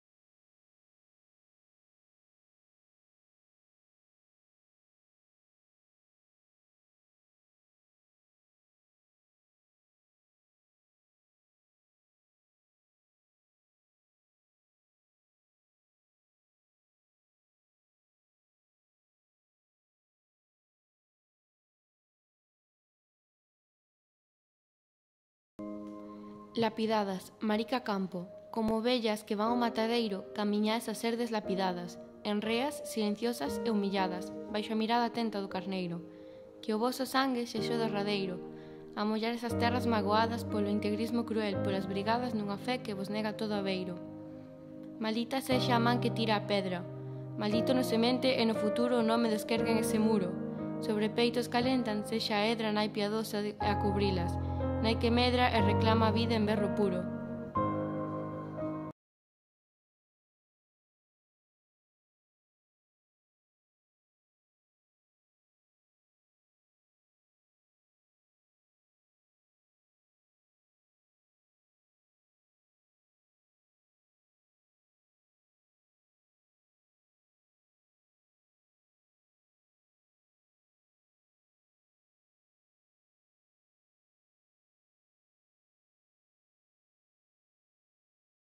LAPIDADAS Ecos de Kim
25N ECOS DE KIM
Unha exposición inspirada en Christine Kim.
Unha maridanza de dúas manifestacións artísticas.
A expresión plástica, obra do alumnado de 4º de ESO,
coas achegas sonoras do alumnado de 3º de ESO.
UN PROXECTO INTERDISCIPLINARIO DOS DEPARTAMENTOS
DE EDUCACIÓN PLÁSTICA E VISUAL
E LINGUA GALEGA E LITERATURA
IES VIRXE DO MAR DE NOIA
Equipo de Igualdade VdM